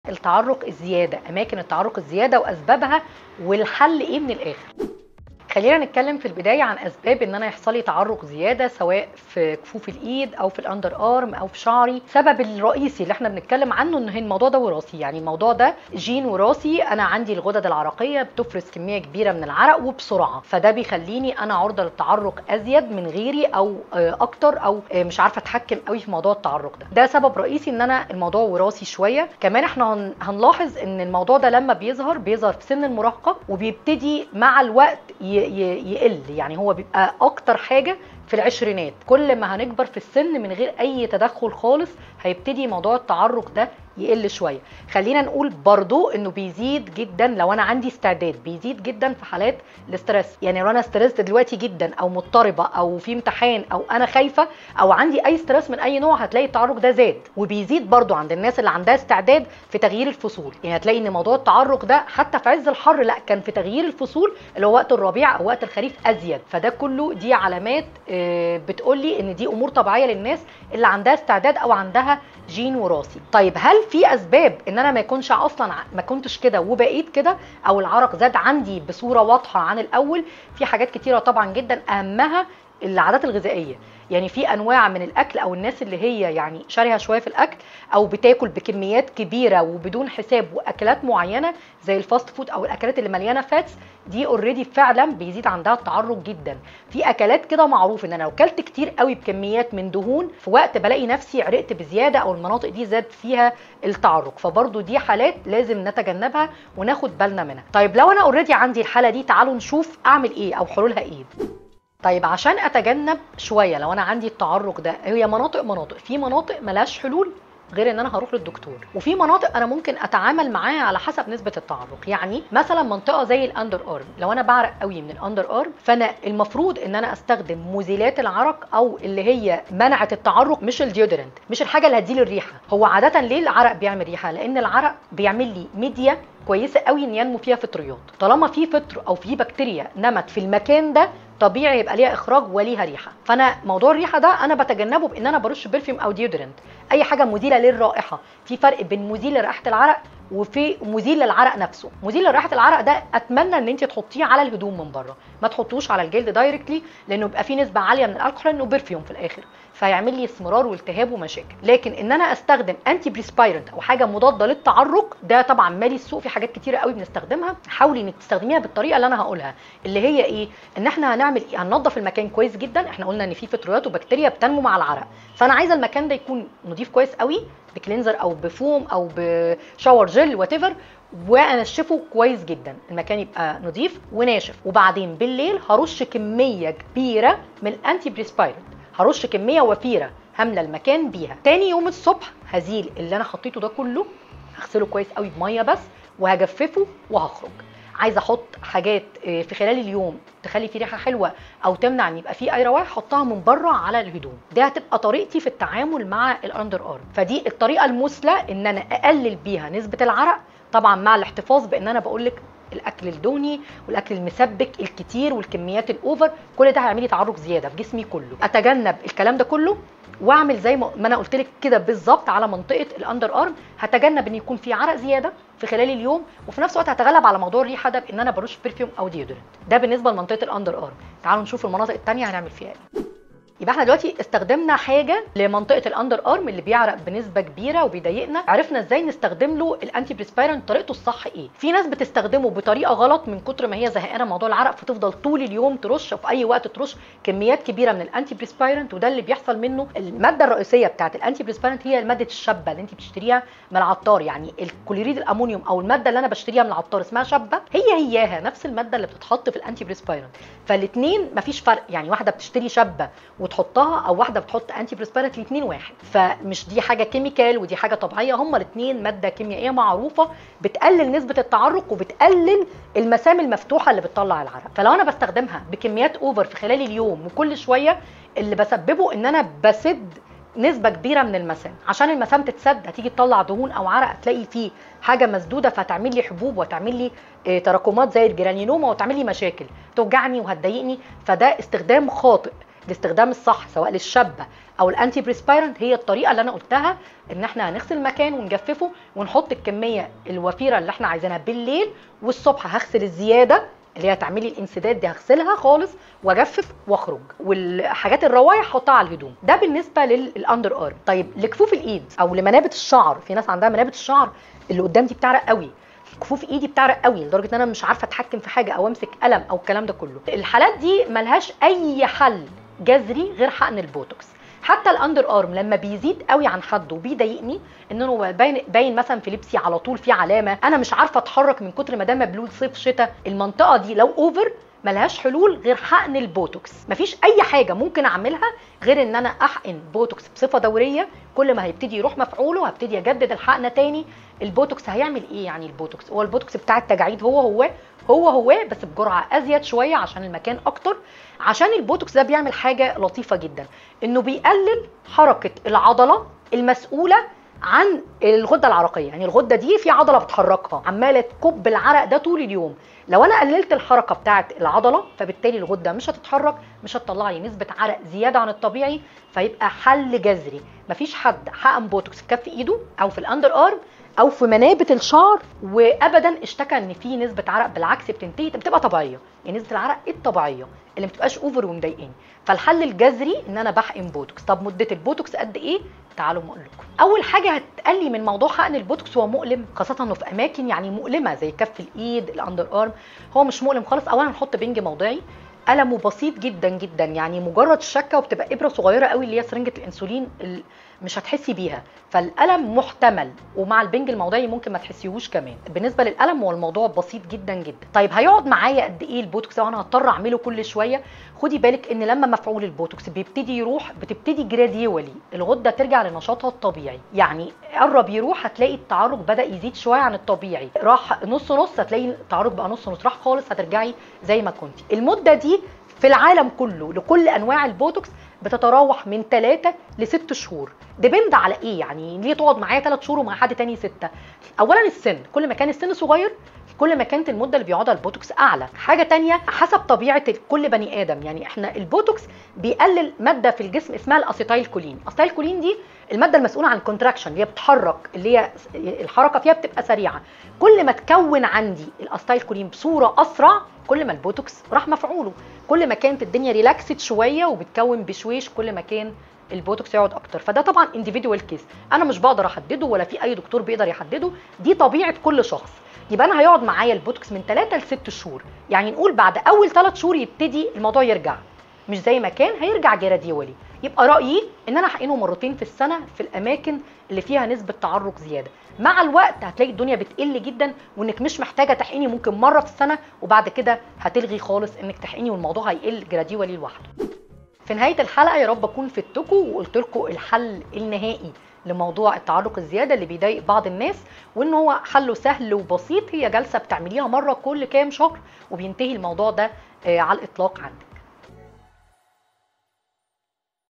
اسباب التعرق الزائد و علاجها | تجنب المواقف المحرجة بسبب التعرق الزائد
مشكلة العرق الزيادة فى بعض المناطق فى الجسم بتسبب لينا الاحراج و التوتر الشديد
د. شيرى محى الدين هتقولك تعالجيها ازاى